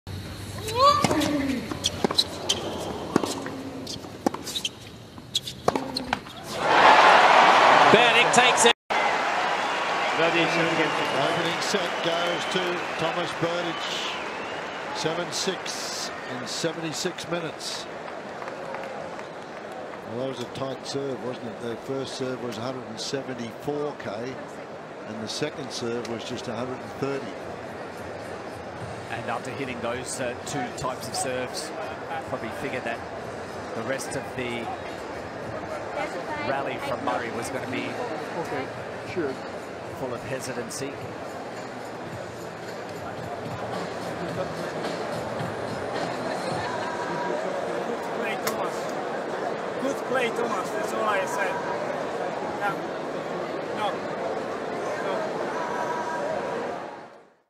Berdych trash talking Murray after Australian Open Semifinal first set
Murray accused Berdych of congratulating him for the unforced error that gave the czech the first set of the match